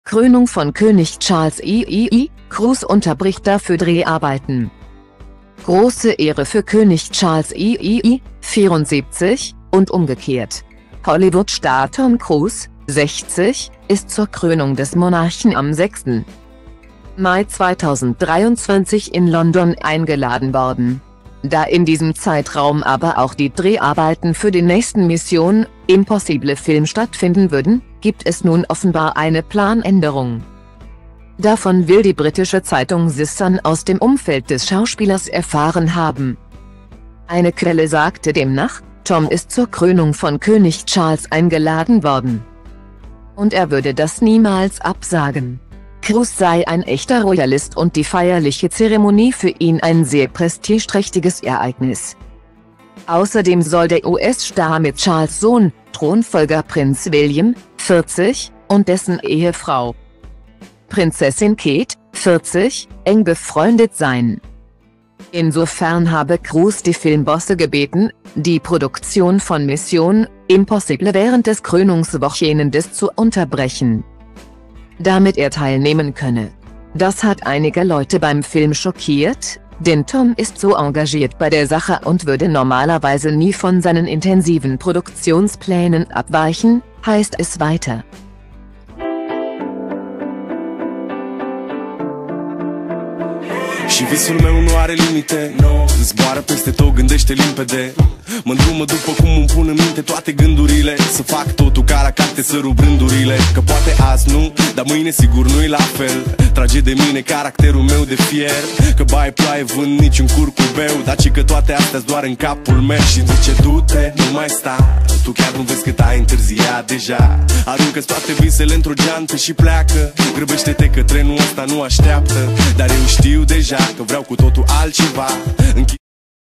Krönung von König Charles III.: Cruise unterbricht dafür Dreharbeiten
Große Ehre für König Charles III. (74) - und umgekehrt. Hollywood-Star Tom Cruise (60) ist zur Krönung des Monarchen am 6.Mai 2023 in London eingeladen worden. Da in diesem Zeitraum aber auch die Dreharbeiten für den nächsten "Mission: Impossible"-Film stattfinden würden, gibt es nun offenbar eine Planänderung.  Davon will die britische Zeitung "The Sun" aus dem Umfeld des Schauspielers erfahren haben.